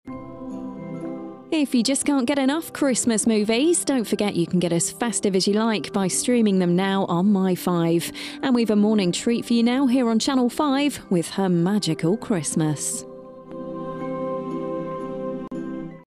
Channel 5 Christmas 2020 ident - Light Green
The second ident to appear for Channel 5's festive presentation, the light green variation.